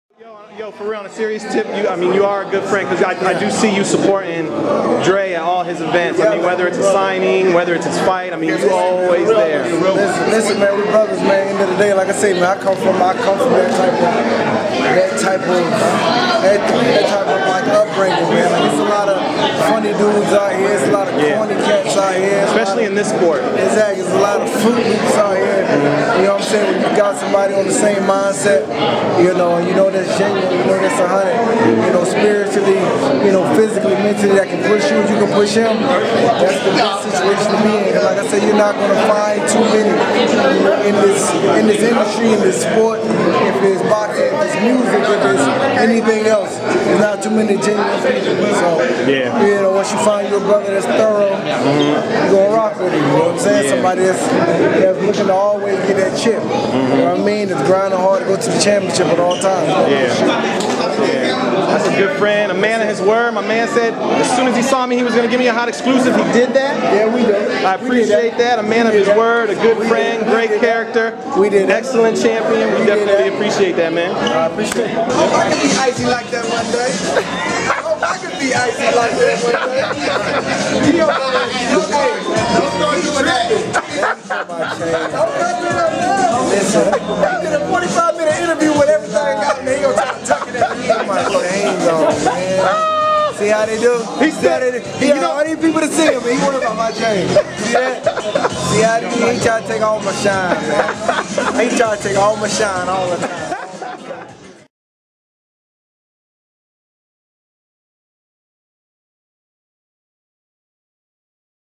ANDRE BERTO EXPLAINS CLOSE FRIENDSHIP WITH ANDRE WARD: "WE BROTHERS...THAT'S GENUINE"
"Listen, man, we brothers...I come from that type of upbringing...it's a lot of funny dudes out there...a lot of corny cats...when you got somebody on the same mindset, you know that's genuine," stated former world champion Andre Berto, who talked about his close friendship with undefeated former world champion Andre Ward. Check it out!